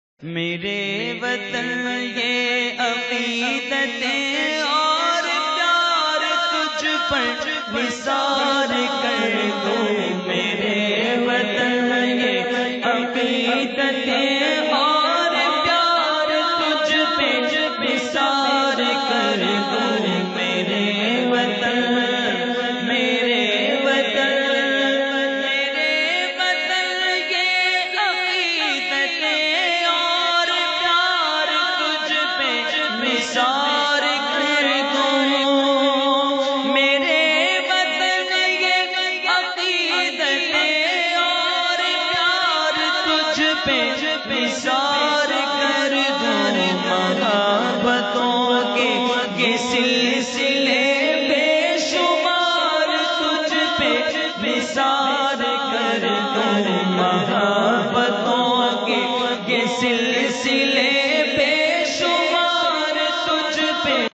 Pakistani Nagma 2019 in Raili Mufti junaid raza khan qadri
independence day of pakistan
14aug 2019